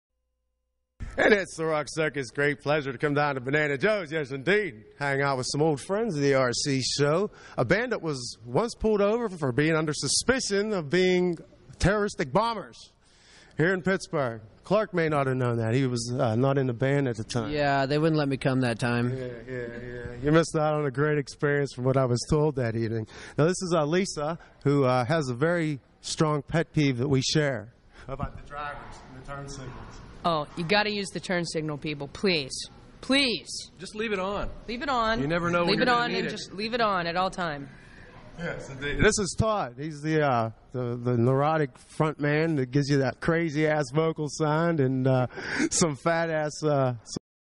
Interview (in the dark) with The Toadies
Although the camera was malfunctioning that day, we carried on with our interview with The Toadies. This was their second appearance on the show, and nearly 10 years between interviews.